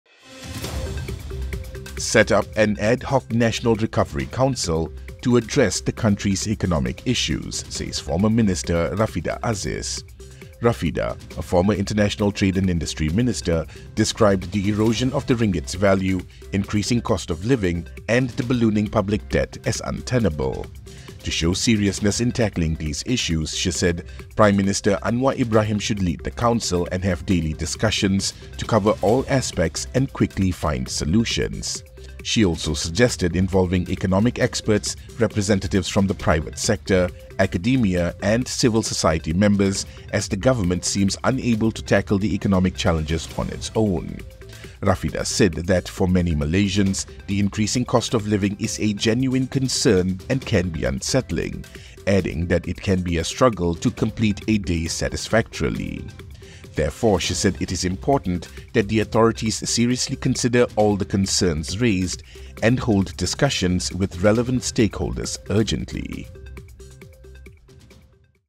Recovery council needed to address economic woes, says Rafidah
The former minister describes the erosion of the ringgit’s value, the increasing cost of living, and the ballooning public debt as untenable.